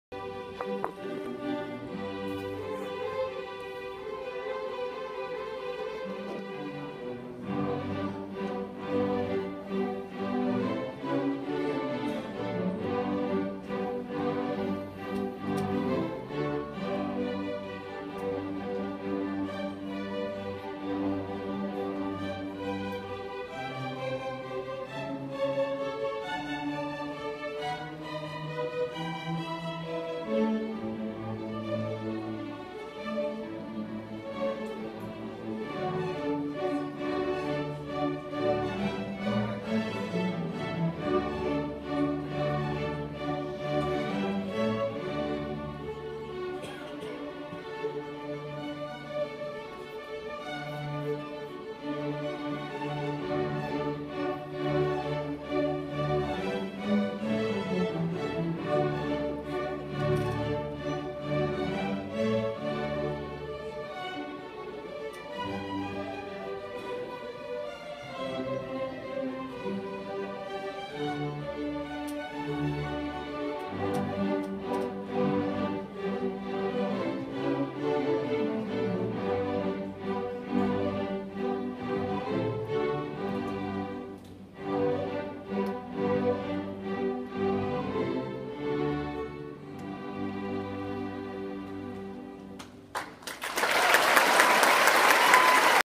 Robert Earl Keen HCYO Benefit Concert 2015 #4
Hill Country Youth Orchestra performs Vivaldi at the annual Robert Earl Keen Benefit Concert for HCYO.